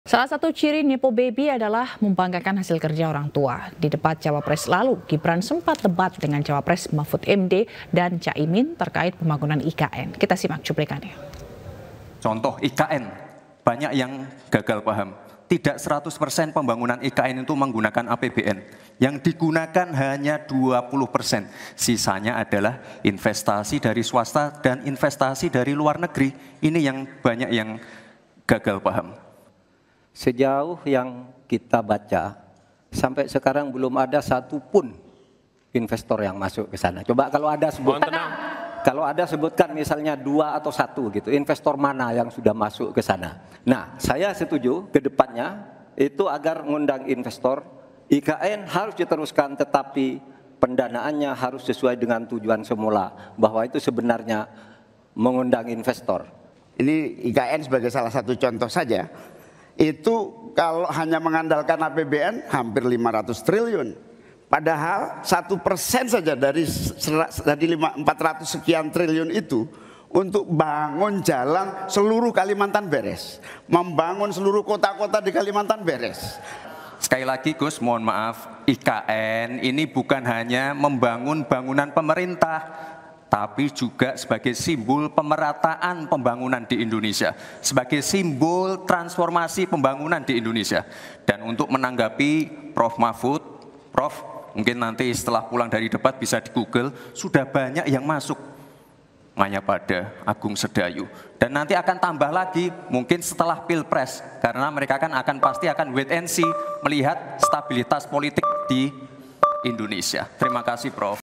Banggakan Kinerja Orang Tua, Gibran Disebut 'Nepo Baby'
Calon wakil presiden (cawapres) Gibran Rakabuming Raka menilai banyak masyarakat gagal paham terkait sumber dana pembangunan Ibu Kota Nusantara (IKN). Dia menegaskan sumber pendanaan IKN tak sepenuhnya dari APBN. Menurutnya, anggaran yang digelontorkan dari kas negara untuk pembangunan IKN cuma 20%. Sisanya dari investasi swasta dan luar negeri.